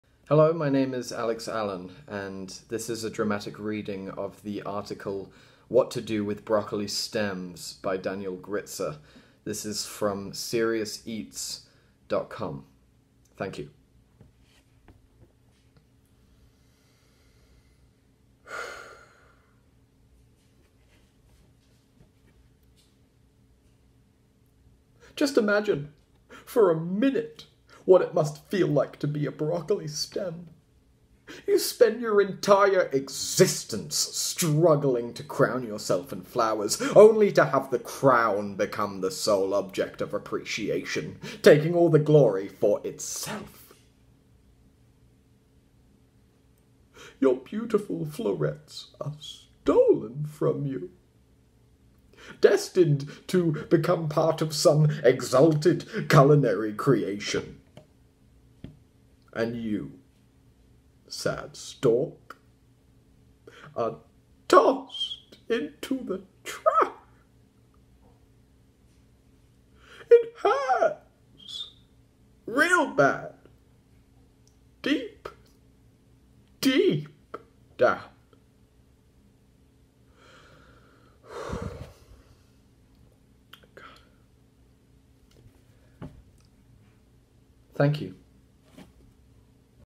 Recipe Readings: Broccoli Stems
Thank you for witnessing my heart and bravery in this piece. More dramatic readings to follow.
Super Cool Oh Yes is a sketch team based in Toronto.